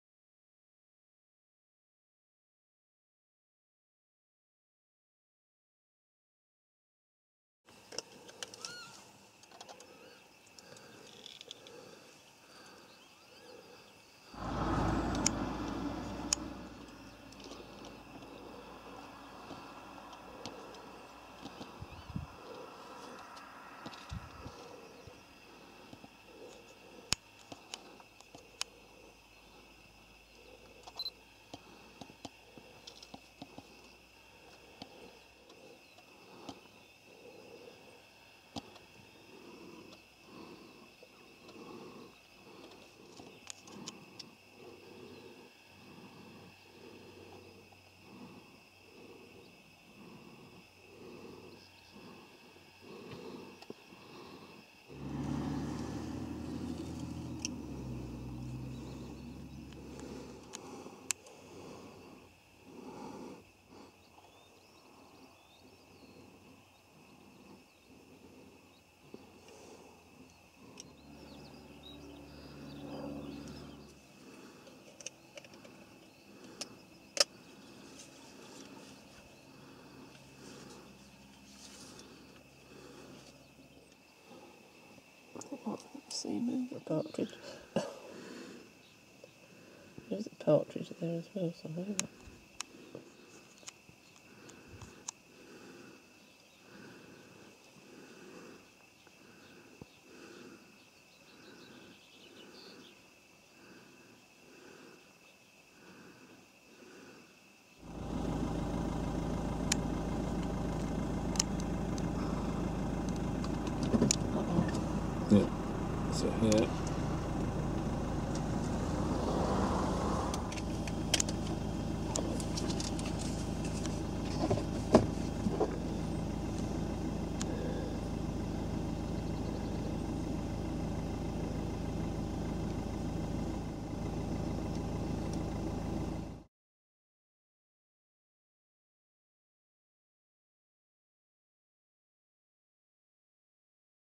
Little Cressingham-Roe Deer, Hares, Partridge & a White Pheasant
Returning home to Watton from a shopping trip to Swaffham this is what we saw in two adjacent fields in Little Cressingham.